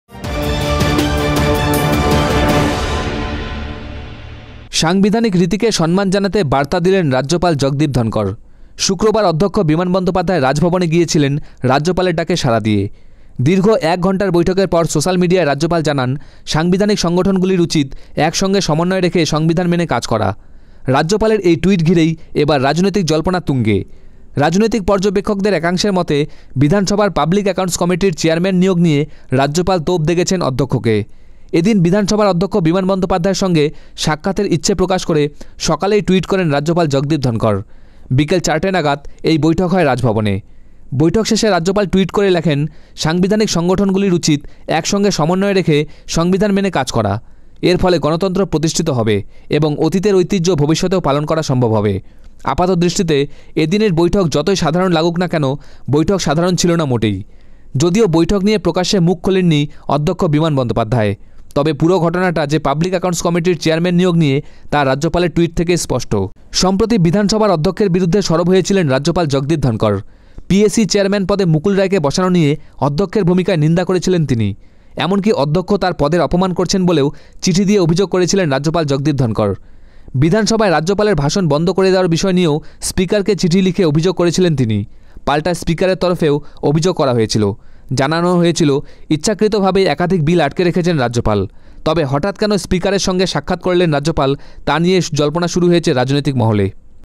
সাংবিধানিক রীতিকে সম্মান করুন, স্পিকারের সঙ্গে বৈঠকের পর রাজ্যপাল | Governor | Speaker | bengalinews
সাংবিধানিক রীতিকে সম্মান করুন, স্পিকারের সঙ্গে বৈঠকের পর রাজ্যপাল | Governor | Speaker | bengalinews | new2newz